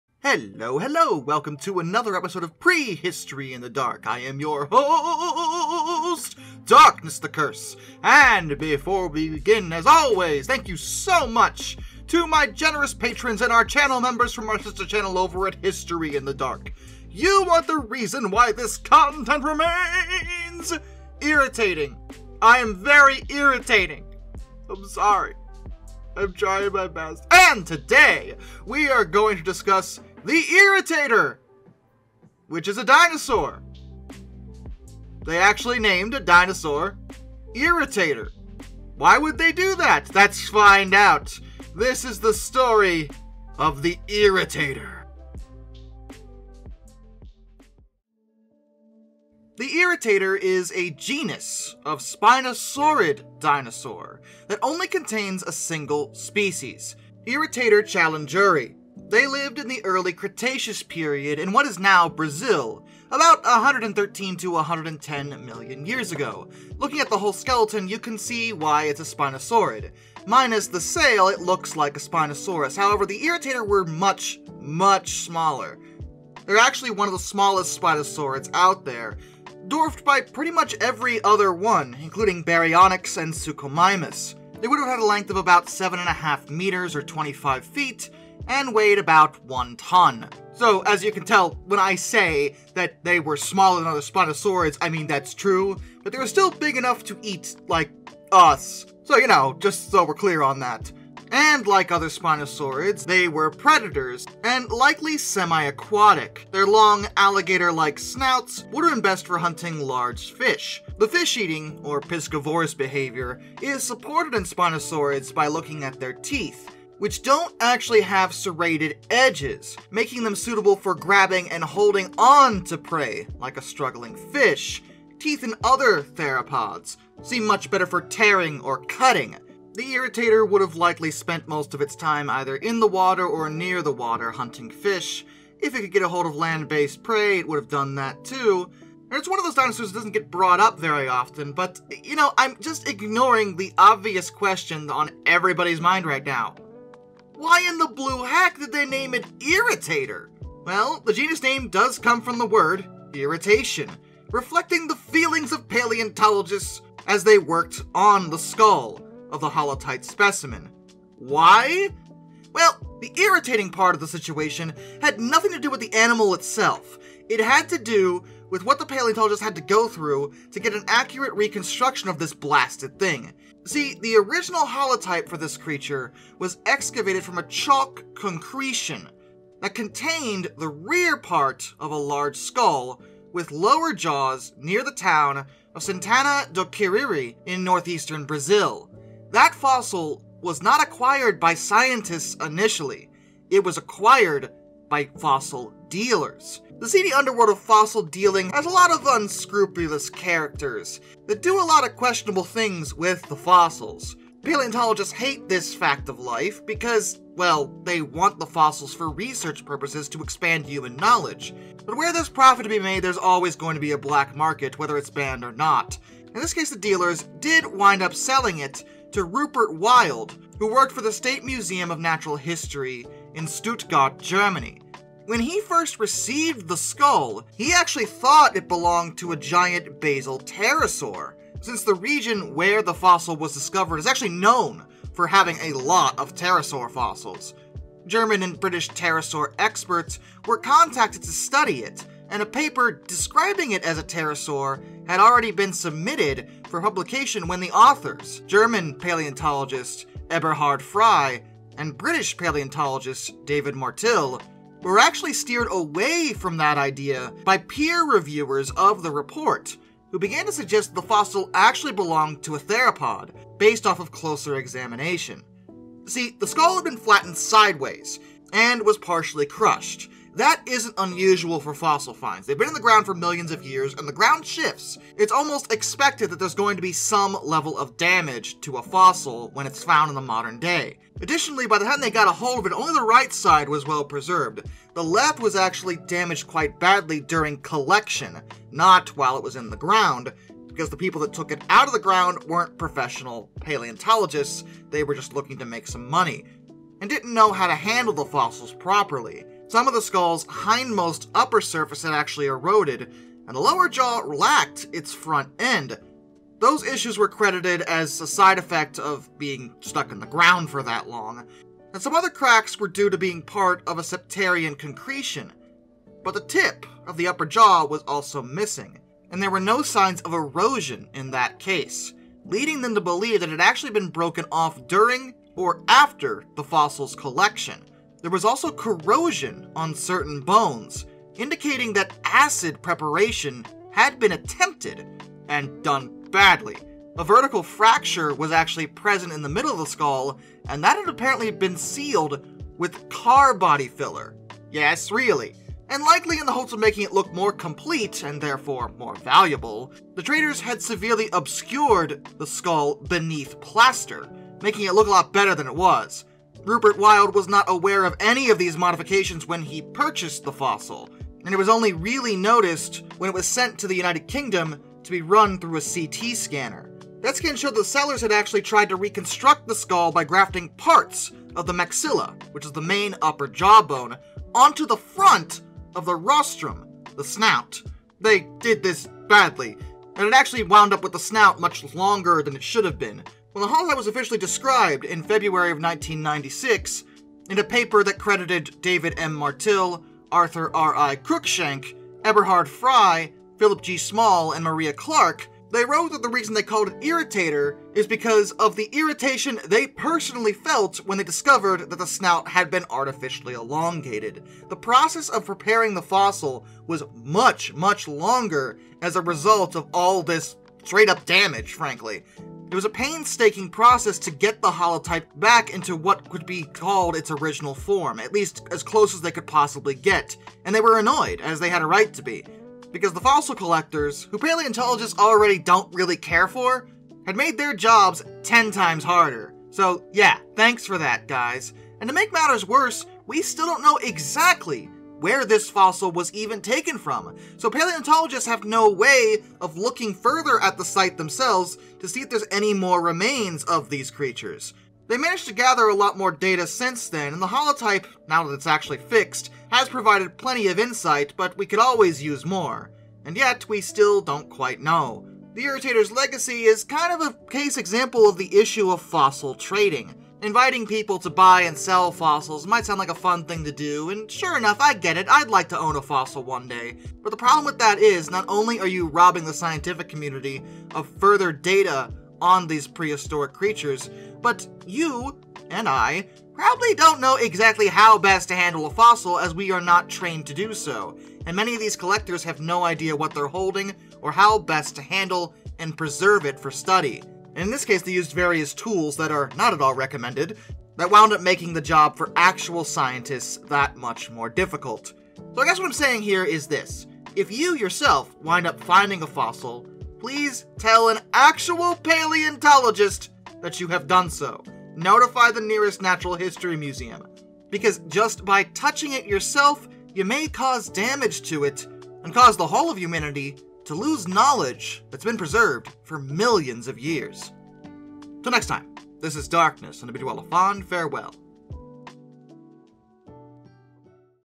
The Dinosaur That Annoyed Paleontologists | Irritator challengeri | Prehistory in the Dark
They bother people a lot.

"Irritator is a genus of spinosaurid dinosaur that lived in what is now Brazil during the Albian stage of the Early Cretaceous Period, about 113 to 110 million years ago. It is known from a nearly complete skull found in the Romualdo Formation of the Araripe Basin. Fossil dealers had acquired this skull and sold it to the State Museum of Natural History Stuttgart. In 1996, the specimen became the holotype of the type species Irritator challengeri. The genus name comes from the word "irritation", reflecting the feelings of paleontologists who found the skull had been heavily damaged and altered by the collectors. The species name is a homage to the fictional character Professor Challenger from Arthur Conan Doyle's novels."